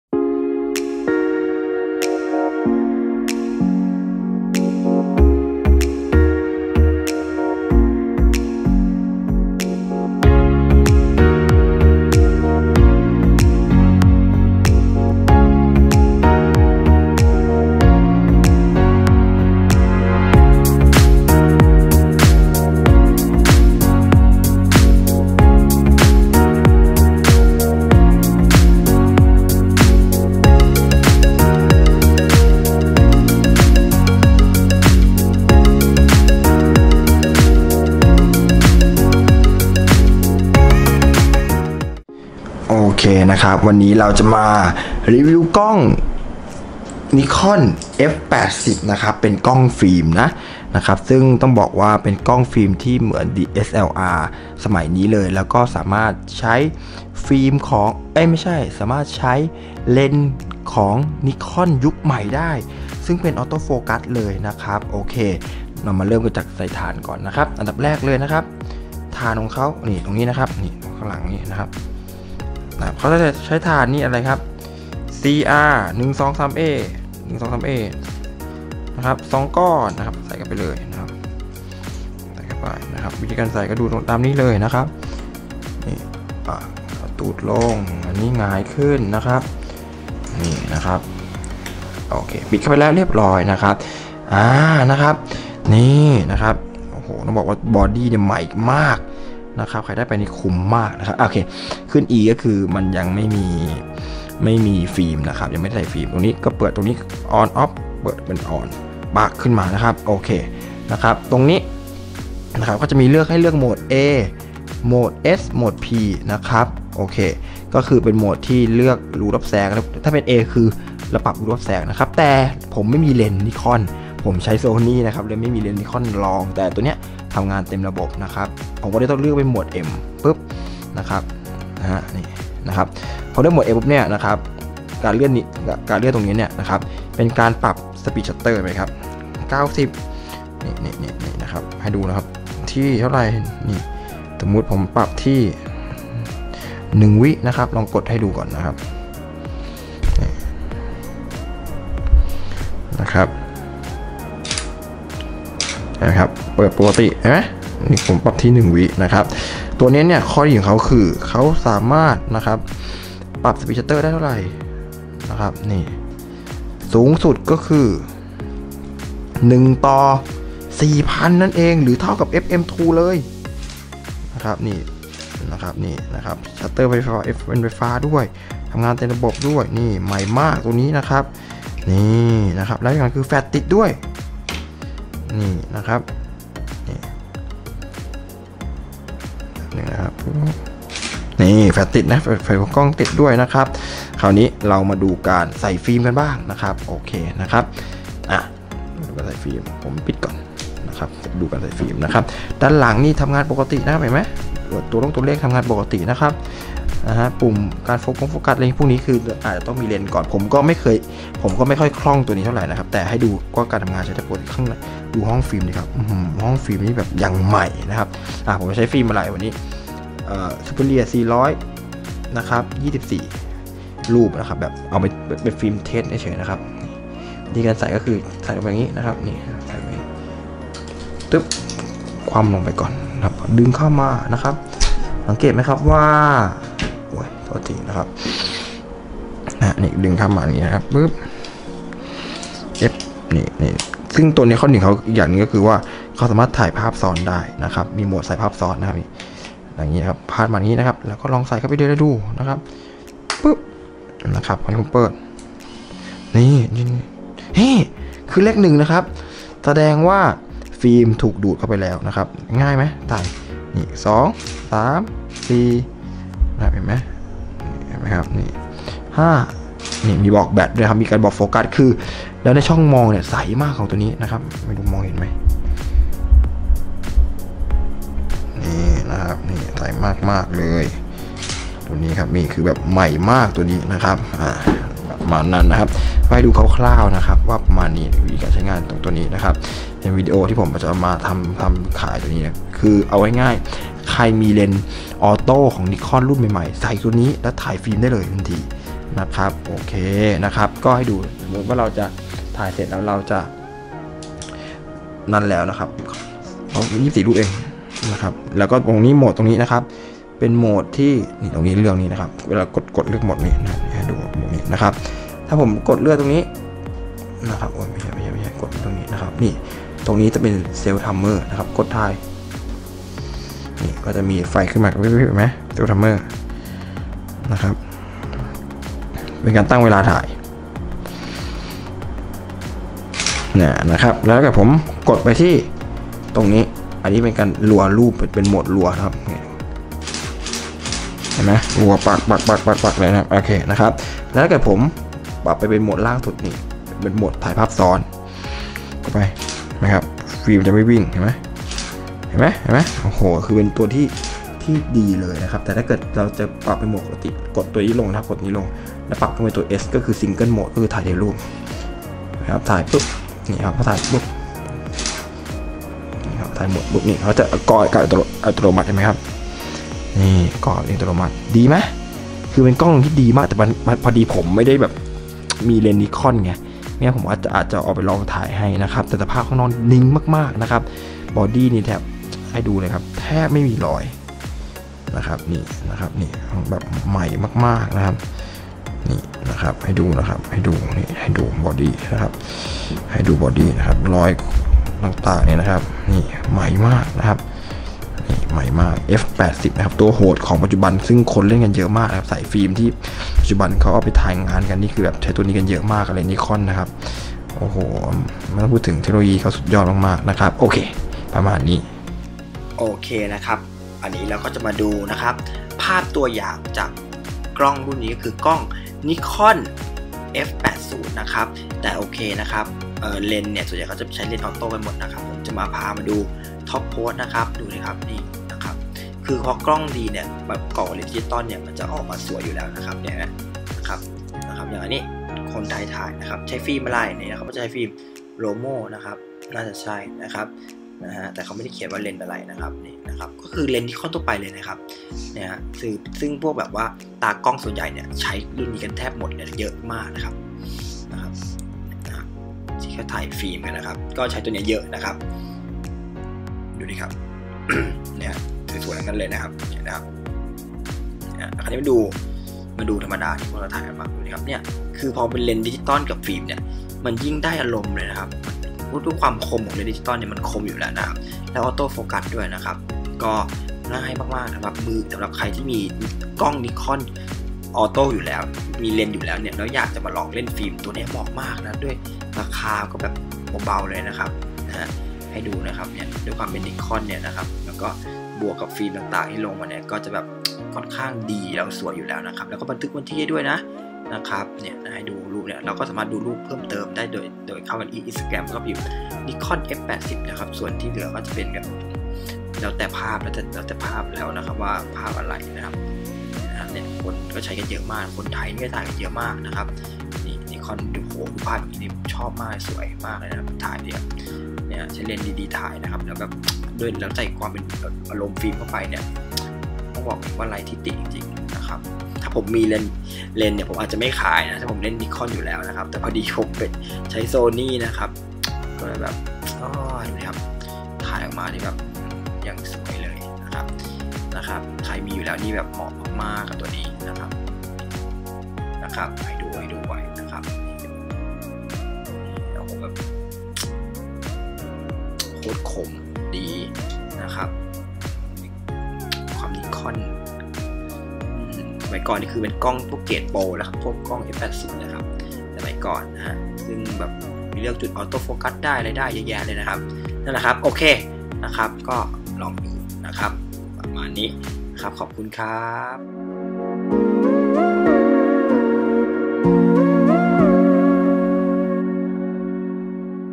รีวิวกล้องฟิล์ม NIKON F80
กล้องฟิล์ม NIKON F80
(NEAR MINT)
ราคา 1500 บาท ส่งฟรีครับ
ใครใช้ กล้องdslr ของnikon อยู่แล้วกำลังจะหากล้องฟิล์มคู่ใจอีกซักตัวลองดูรุ่นนี้เลยครับไม่มีรุ่นใหนเหมาะสมเท่าแล้วครับ สามารถใช้เลนส์ Nikon F mount Type D และ G รวมถึงระบบ vr (กันสั่น)ด้วย มีโหมดถ่ายภาพแบบ M/A/S/P มีระบบวัดแสง 3 แบบ มีระบบถ่ายคร่อม, ถ่ายซ้อน มีปุ่มล็อคค่าแสง ล็อคโฟกัส ระบบโฟกัสแบบต่อเนื่อง สปีตชัตเตอร์สูงสุดถึง 1/4000
เป็นรุ่นที่ตากล้องใช้ถ่ายงานกันเป็นจำนวนมากครับ
สภาพภายนอก 99%
ทำงานเต็มระบบ
แฟลชหัวกล้องติด
ช่องมองใสปิ๊ง

ดูรีวิวเบื้องต้นในคลิปนะครับ

(ตัวในคลิปคือตัวเดียวกับที่ขายครับ)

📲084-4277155
line : diawdiamond